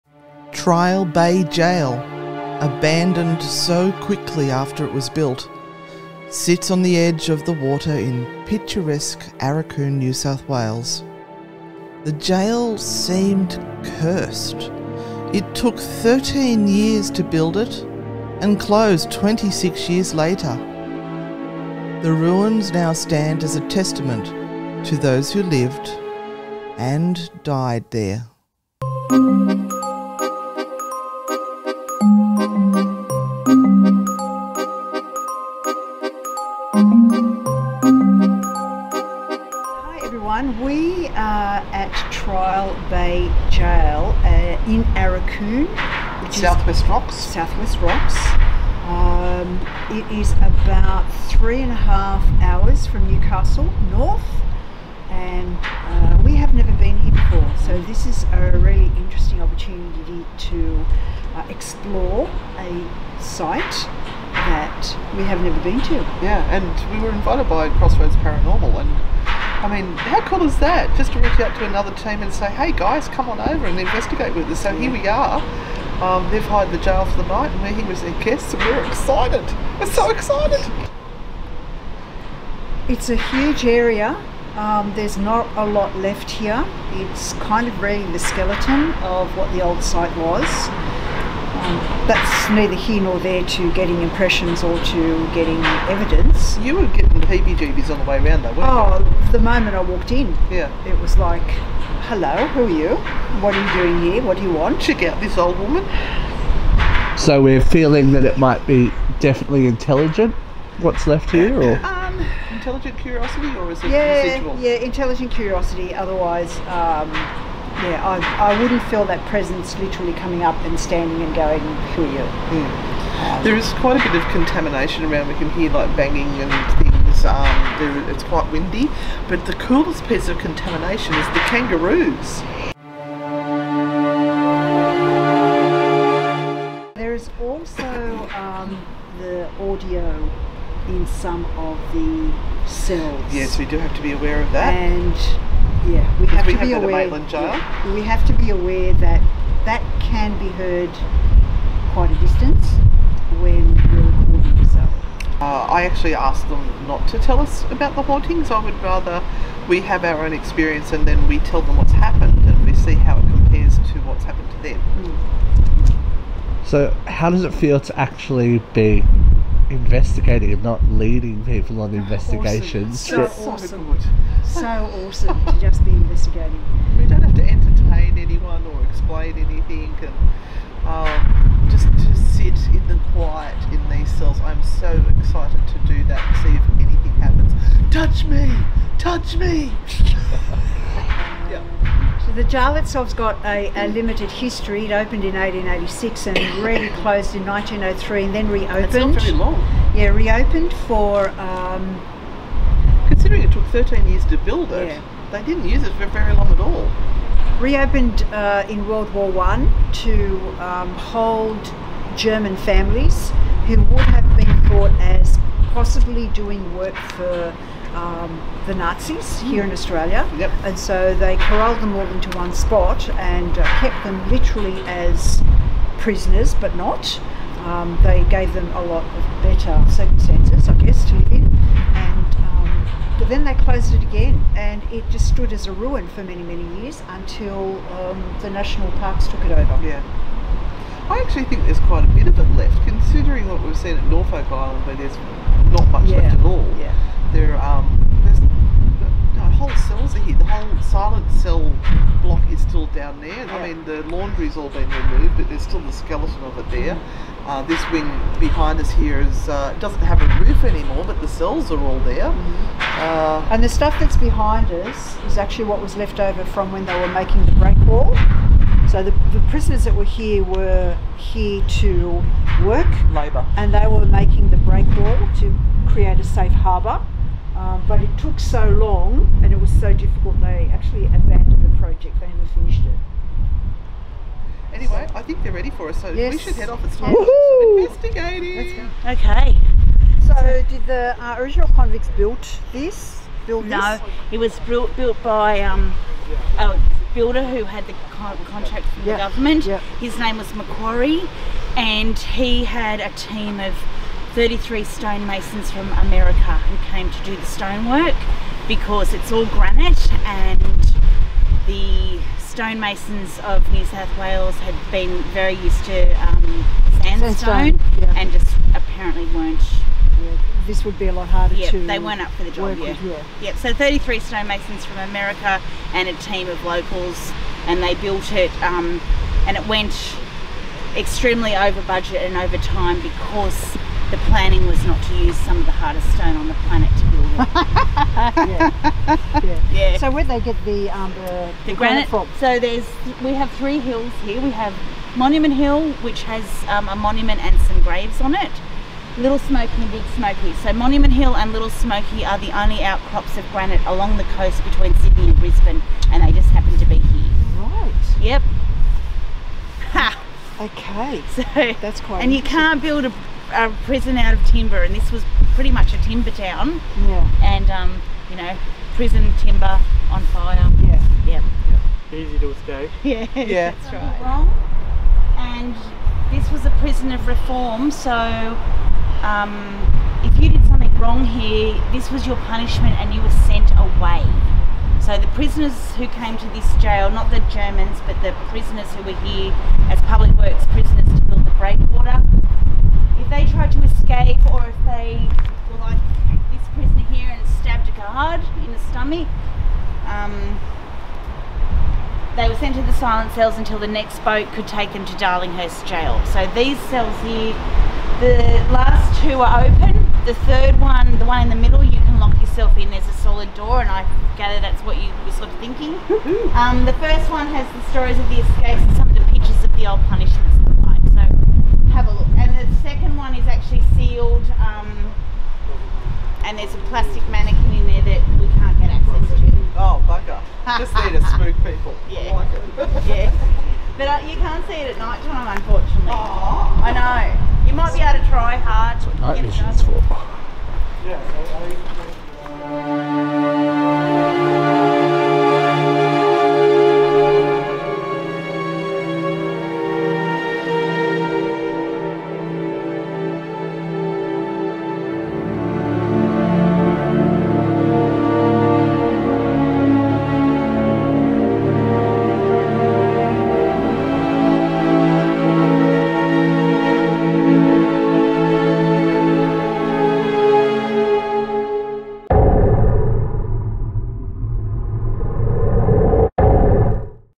Ghost Grannies Ghost Hunting adventure | Road Trip with Cousin Steve | Part 2 | Trial Bay Gaol NSW
Ghost Grannies, Anne & Renata, and Cousin Steve as they begin their investigation of Trial bay Gaol, Arakoon, NSW Australia.
We find out about the history of the place, the crazy creatures that roam there, and finally get to explore a haunted location.
Many thanks to Geoff and the team from The Crossroads  for their invitation to join them for the night

Paranormal content, special guests, spooky stories and general shenanigans. We want to bring back the fun in paranormal!